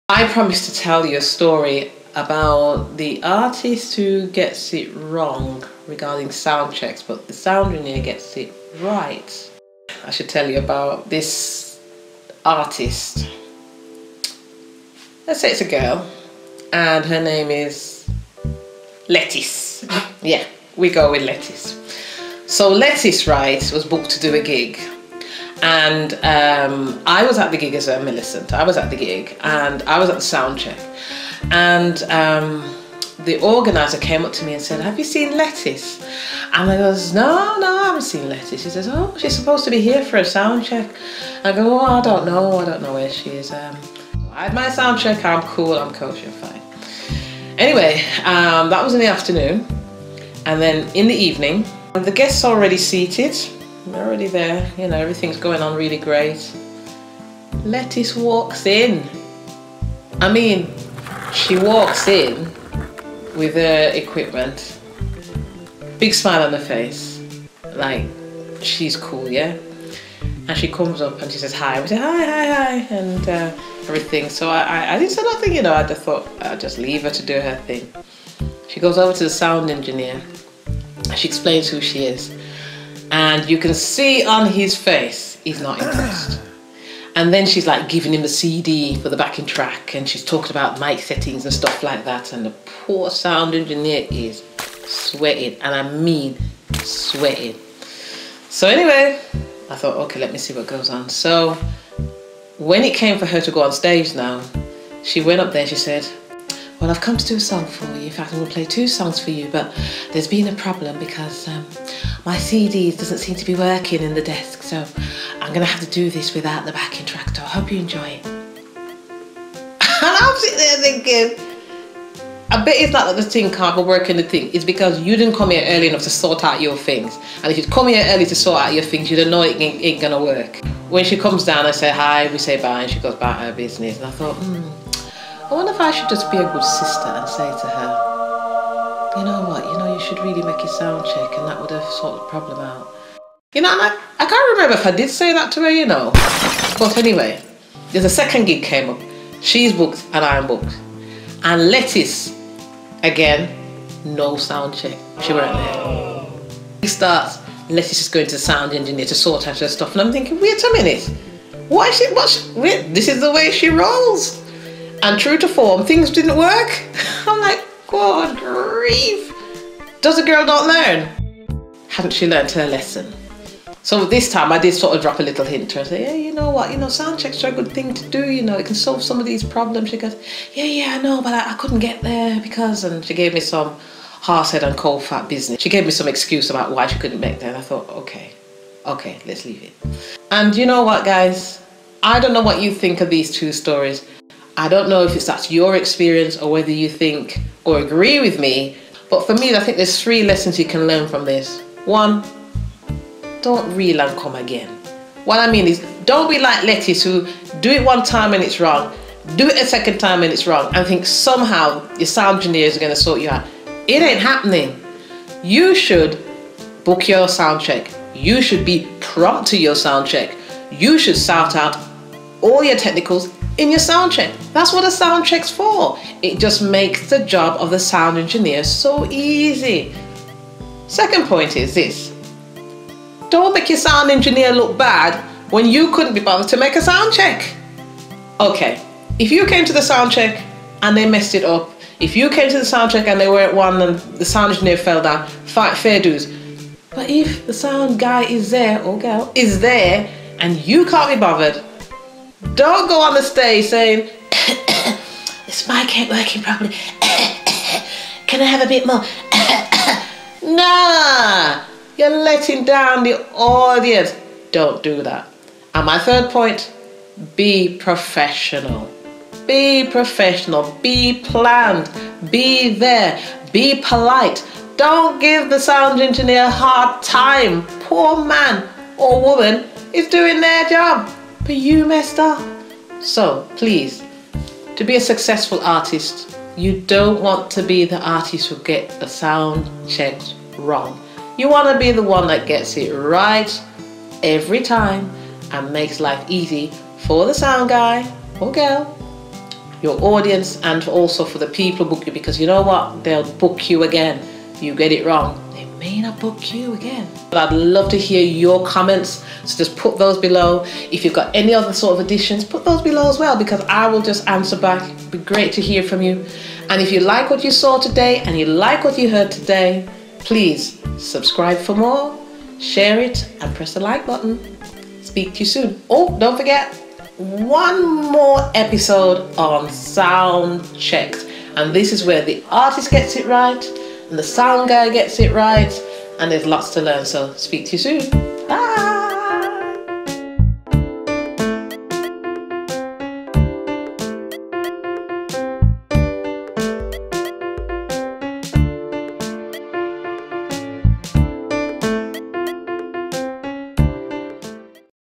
[STORY TIME] Sound Guys - She Got It Wrong
Sound checks: What do you do when the Sound Engineer gets it right but the Artiste gets it wrong?

Video production by Sun.REI